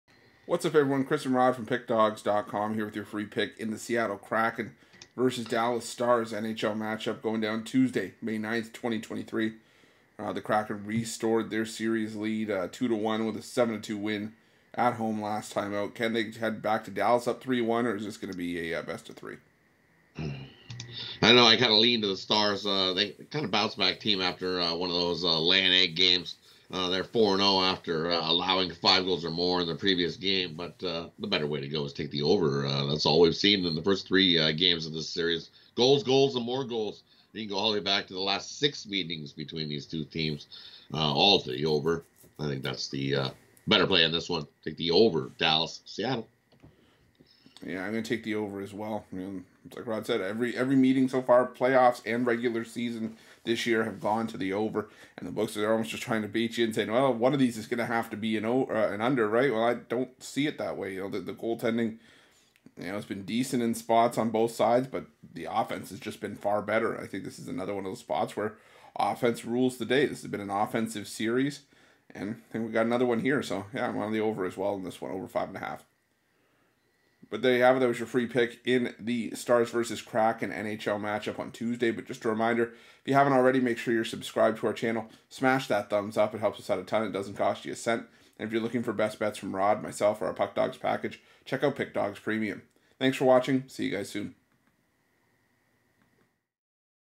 Seattle Kraken vs Dallas Stars 5/9/23 NHL Free Pick Free NHL Betting Tips
The Seattle Kraken face the Dallas Stars in Tuesday's NHL action. Get The Puckdawgz' NHL Pick and Prediction for this game.